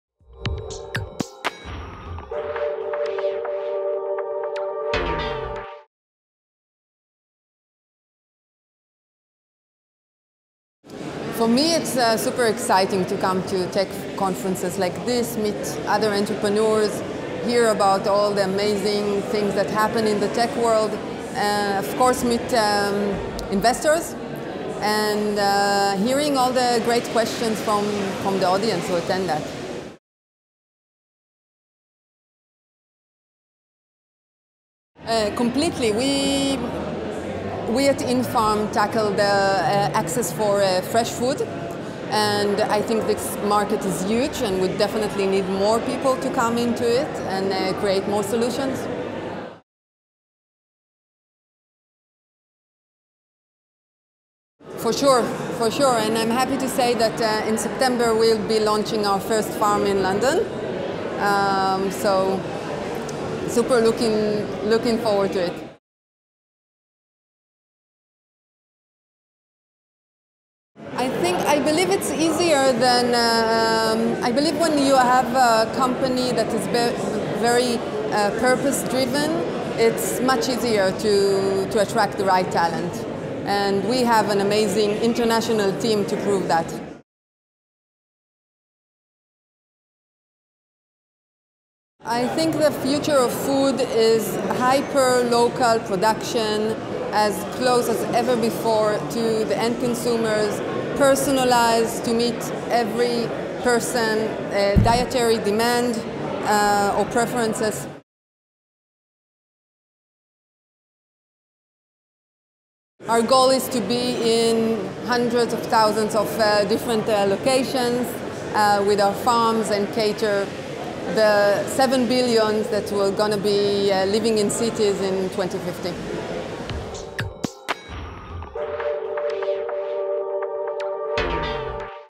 Interview with Osnat Michaeli | STYT
infarm Co-Founder and Chief Brand Officer Osnat Michaeli discusses opportunities for tech companies to impact the community at Bloomberg’s global technology summit Sooner Than You Think in London.